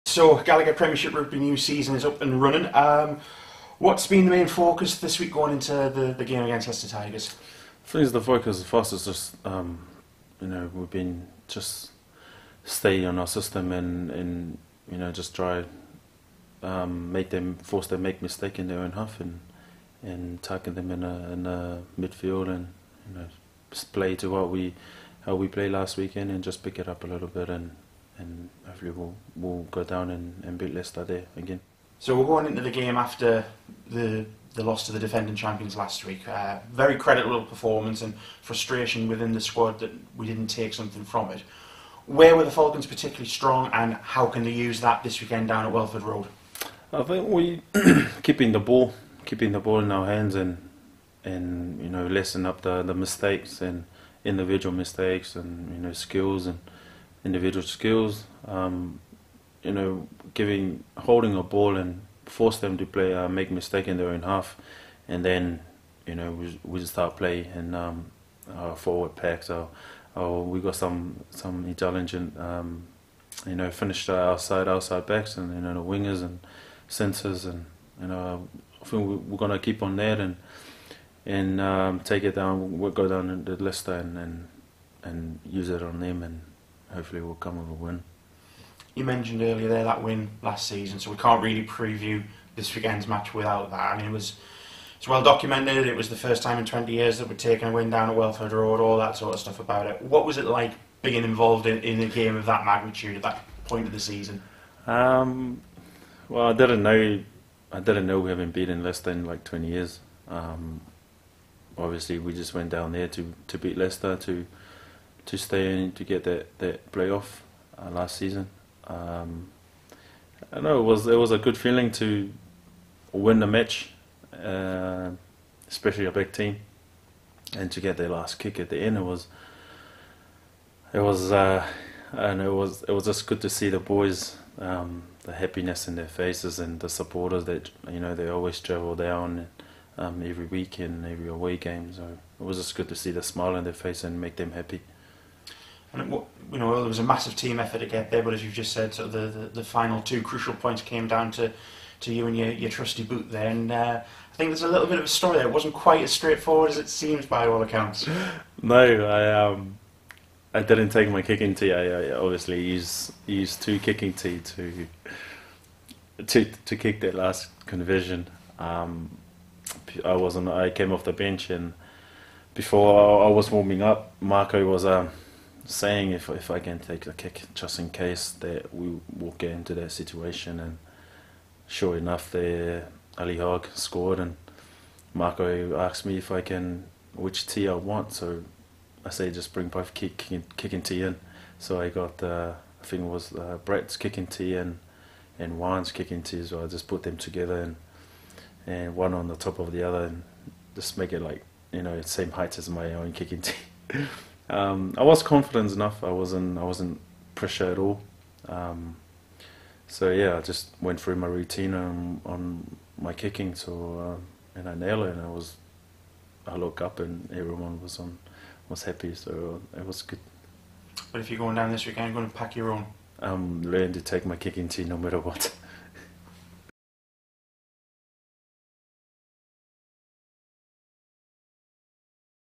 Player Preview - Leicester away
Sonatane Takulua, the man who kicked the winning points on the club's last visit to Welford Road, speaks to Falcons TV ahead of the Gallagher Premiership Rugby away fixture against Leicester Tigers.